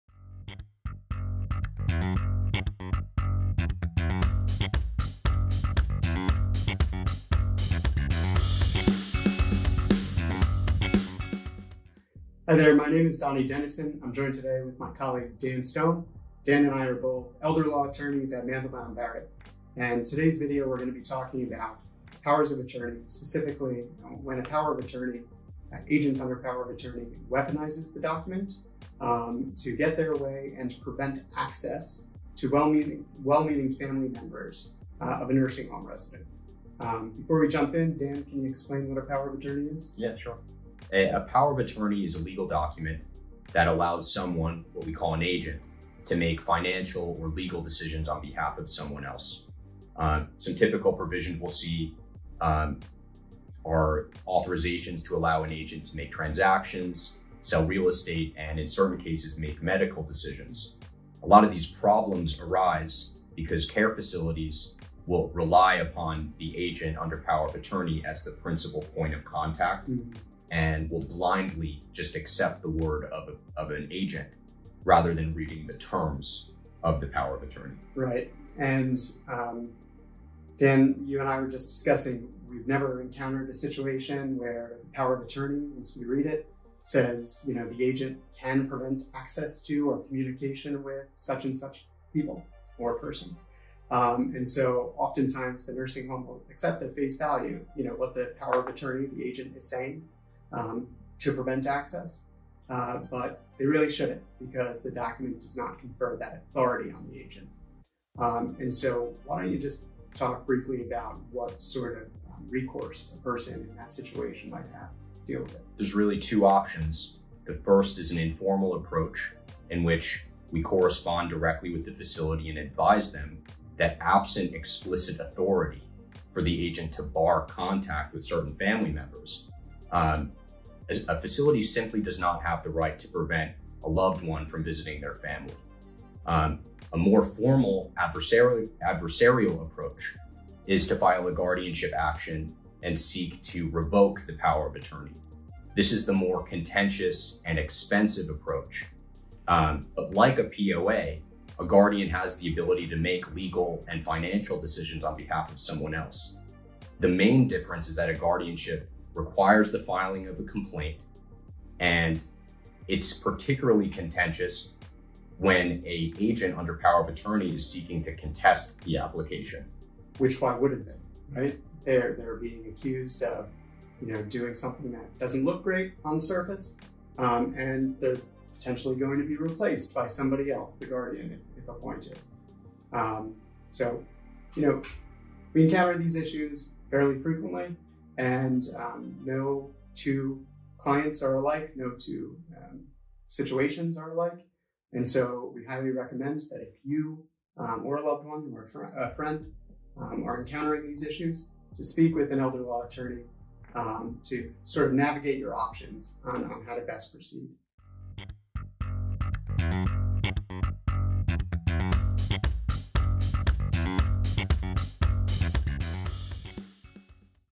Exploitation of Power of Attorney: Agent Blocking Family Access to Nursing Home Resident
In their latest vlog, Mandelbaum Barrett PC Elder Law attorneys, Donald Dennison, Esq. and Dan Stone, Esq. discuss the implications of a Power of Attorney agent weaponizing the document to prevent well-meaning family members from visiting a nursing home resident.